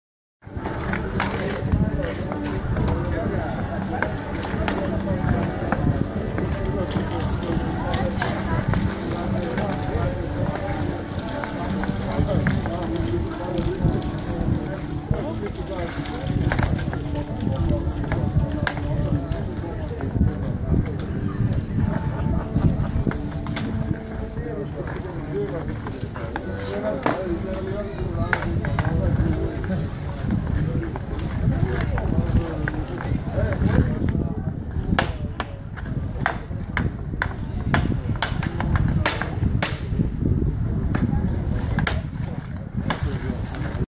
FIDAice - test mladih hokejistov in umetnostne drsalke
V okviru praznovanja svetovnega dneva snega 2016 v Ljubljani, so na majhnem drsališču FIDAice užival mladi hokejisti. Vmes se jim je pridružila umetnostna drsalka.

FIDAice - najboljša drsališča iz umetne mase

Drsajte 365 dni v letu. Brez mraza, brez elektrike, brez vode!

Narejena v Sloveniji.